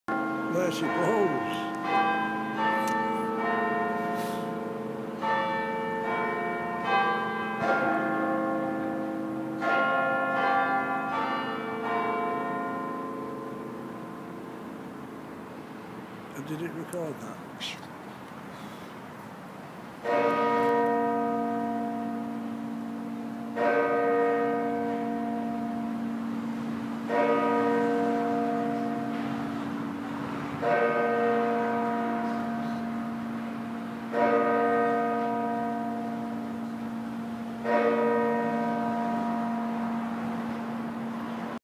Rochdale's Big Ben, what a beauty
Rochdale Town Hall chimes tonight at 6pm. I grew up with this.